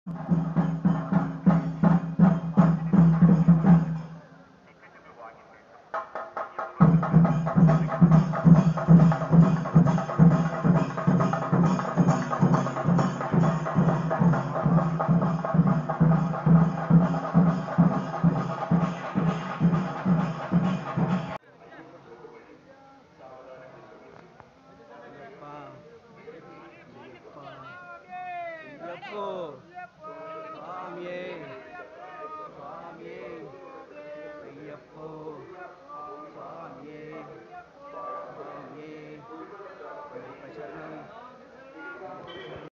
PAMBA, way to shabarimala.
pamba, and pamba river.this is an important place to shabarimala pilgrimage, @eatsetgo2550